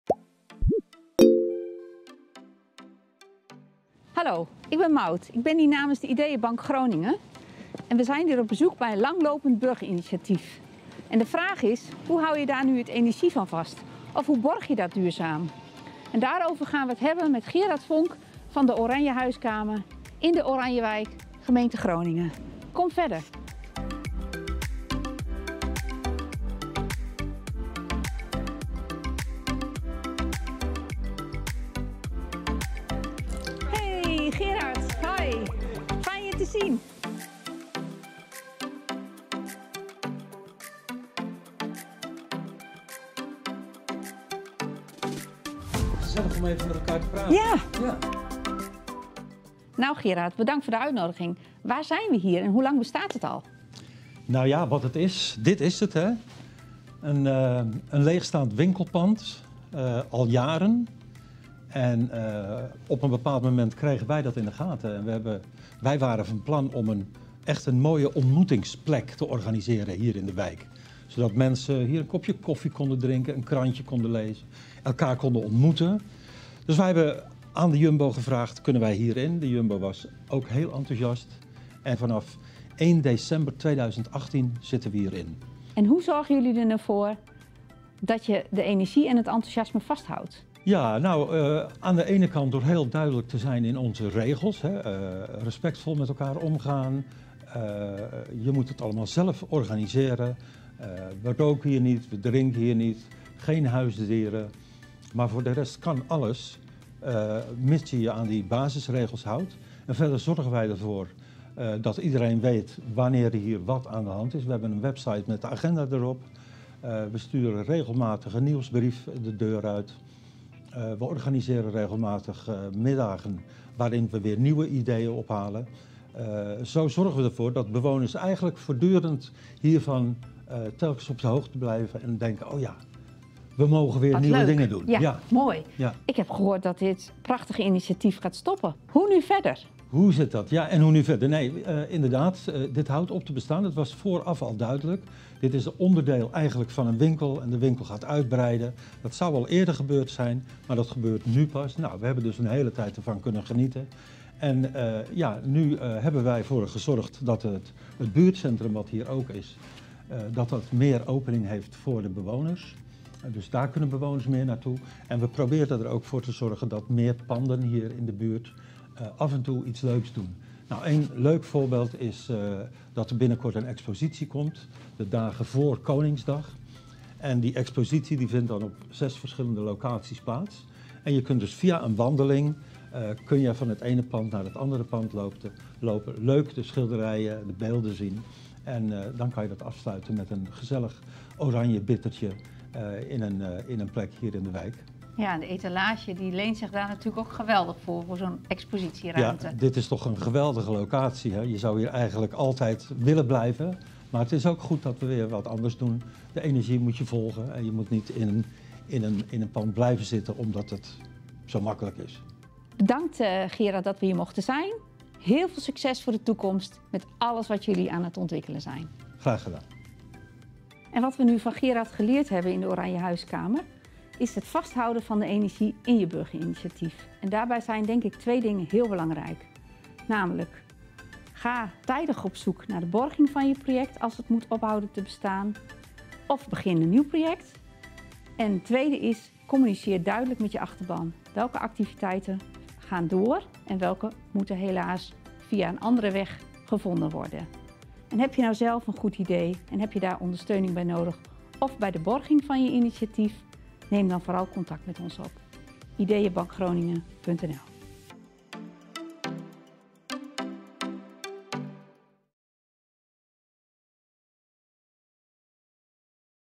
Op bezoek bij de Oranje huiskamer in de Oranjewijk in Groningen
‘Hoe houd je de energie van een langlopend burgerinitiatief vast en hoe borg je dat duurzaam?’
Namens Ideeënbank Groningen ging Maud op bezoek bij de Oranje huiskamer in de Oranjewijk in Groningen. Maud ging in gesprek met Gerard Vonk een van de initiatiefnemers van de Oranje Huiskamer.
Benieuwd!? Kijk dan het filmpje.